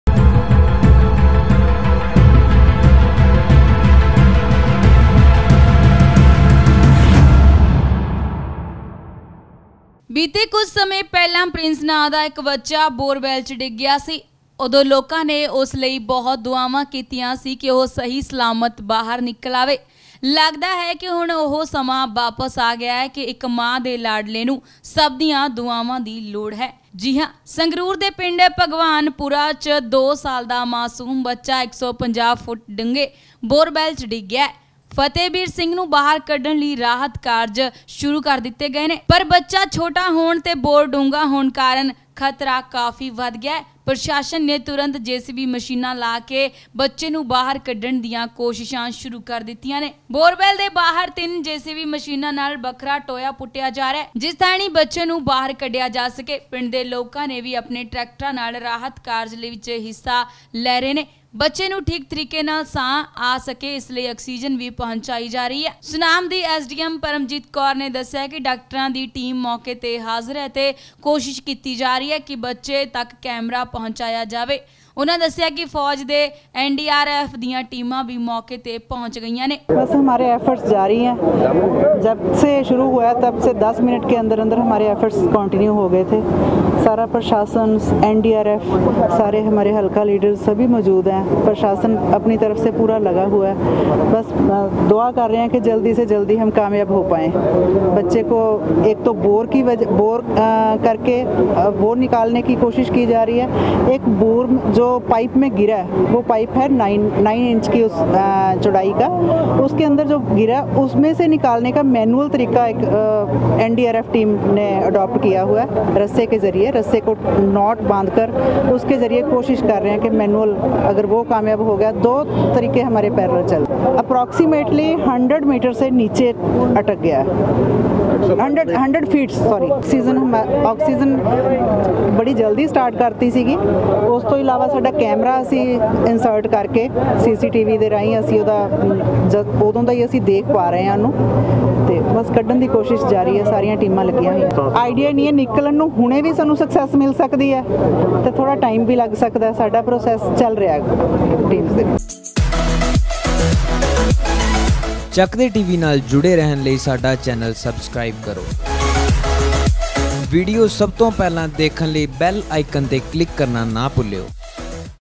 ਪ੍ਰਸ਼ਾਸਨ ਨੇ ਤੁਰੰਤ JCB Machines ਲਾ ਕੇ ਬੱਚੇ ਨੂੰ ਬਾਹਰ ਕੱਢਣ ਦੀਆਂ ਕੋਸ਼ਿਸ਼ਾਂ ਕੀਤੀਆਂ ਸ਼ੁਰੂ
ਬੀਤੇ ਕੁੱਝ ਸਮੇਂ ਪਹਿਲਾਂ ਪਿੰਸ਼ ਨਾਂ ਦਾ ਇੱਕ ਬੱਚਾ ਬੋਰਵੈੱਲ 'ਚ ਡਿੱਗ ਗਿਆ ਸੀ ਉਦੋ ਲੋਕਾਂ ਨੇ ਉਸ ਲਈ ਬਹੁੱਤ ਦੁਅਵਾਂ ਕੀਤੀਆਂ ਸੀ ਕਿ ਉਹ ਸਹੀ ਸਲਾਮਤ ਬਾਹਰ ਨਿਕਲ ਆਵੇ…. ਲ਼ੱਗਦਾ ਹੈ ਕਿ ਹੁਣ ਉਹ ਸਮਾਂ ਫੇਰ ਵਾਪਸ ਆ ਗਿਆਂ ਕਿ ਇੱਕ ਮਾਂ ਦੇ ਲਾਡਲੇ ਨੂੰ ਸਭ ਦੀਆਂ ਦੁਆਵਾ ਦੀ ਲੋੜ ਹੈ ਜੀ ਹਾਂ ਸੰਗਰੂਰ ਦੇ ਪਿੰਡ ਭਗਵਾਨਪੁਰਾ ਵਿੱਚ ਦੋ ਸਾਲ ਦਾ ਮਾਸੂਮ ਬੱਚਾ ੧੫੦ ਫੁੱਟ ਡੂੰਘੇ ਬੋਰਵੈੱਲ ਵਿੱਚ ਡਿੱਗ ਗਿਆ ਹੈ..ਫ਼ਤਹਿਵੀਰ ਸਿੰਘ ਨੂੰ ਬਾਹਰ ਕੱਢਣ ਲਈ ਰਾਹਤ ਕਾਰਜ ਤਾਂ ਸ਼ੁਰੂ ਹੋ ਗਏ ਨੇ, ਪਰ ਬੱਚਾ ਛੋਟਾ ਹੋਣ ਤੇ ਬੋਰ ਡੂੰਘਾ ਹੋਣ ਕਾਰਨ ਖ਼ਤਰਾ ਕਾਫੀ ਵੱਧ ਹੈ..ਪ੍ਰਸ਼ਾਸਨ ਨੇ ਤੁਰੰਤ ਜੇਸੀਬੀ ਮਸ਼ੀਨਾਂ ਲਾ ਕੇ ਬੱਚੇ ਨੂੰ ਬਾਹਰ ਕੱਢਣ ਦੀਆਂ ਕੋਸ਼ਿਸ਼ਾਂ ਸ਼ੁਰੂ ਕਰ ਦਿੱਤੀਆਂ ਸੀ

ਬੋਰਵੈੱਲ ਦੇ ਬਰਾਬਰ ਤਿੰਨ ਜੇਸੀਬੀ ਮਸ਼ੀਨਾਂ ਨਾਲ ਵੱਖਰਾ ਟੋਇਆ ਪੁੱਟਿਆ ਜਾ ਰਿਹਾ ਹੈ, ਜਿਸ ਥਾਣੀ ਬੱਚੇ ਨੂੰ ਬਾਹਰ ਕੱਢਿਆ ਜਾ ਸਕੇ। ਪਿੰਡ ਦੇ ਲੋਕਾਂ ਨੇ ਵੀ ਆਪਣੇ ਟਰੈਕਟਰਾਂ ਨਾਲ ਰਾਹਤ ਕਾਰਜ ਵਿੱਚ ਹਿੱਸਾ ਲੈ ਰਹੇ ਹਨ।  ਬੱਚੇ ਨੂੰ ਠੀਕ ਤਰੀਕੇ ਨਾਲ ਸਾਹ ਆ ਸਕੇ ਇਸ ਲਈ ਆਕਸੀਜਨ ਵੀ ਪਹੁੰਚਾਈ ਜਾ ਰਹੀ ਹੈ…….ਸੁਨਾਮ ਦੀ ਐਸਡੀਐਮ ਮਨਜੀਤ ਕੌਰ ਨੇ ਦੱਸਿਆ ਕਿ ਡਾਕਟਰਾਂ ਦੀ ਟੀਮ ਮੌਕੇ 'ਤੇ ਹਾਜ਼ਰ ਹੈ ਅਤੇ ਕੋਸ਼ਿਸ਼ ਕੀਤੀ ਜਾ ਰਹੀ ਹੈ ਕਿ ਬੱਚੇ ਤਕ ਕੈਮਰਾ ਪਹੁੰਚਾਇਆ ਜਾਵੇ। ਉਨ੍ਹਾਂ ਦੱਸਿਆ ਕਿ ਫ਼ੌਜ ਤੇ ਐਨਡੀਆਰਐਫ ਦੀਆਂ ਟੀਮਾਂ ਵੀ ਮੌਕੇ 'ਤੇ ਪਹੁੰਚ ਰਹੀਆਂ ਹਨ….